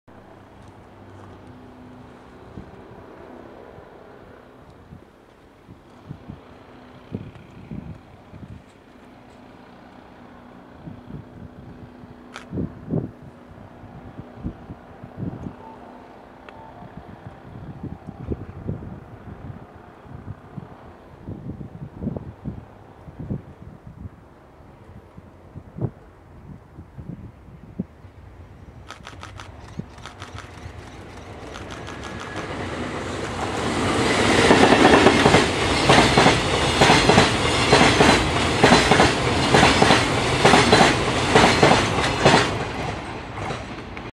【貨物列車】高速貨物3079列車(新)EF65-1121 (青プレ・国鉄原色)
1月28日(土)　讃岐塩屋駅にて
この日の3079レの釜は、(新)EF65-1121(青プレ・国鉄原色)でした！
久々の四国運用充当です！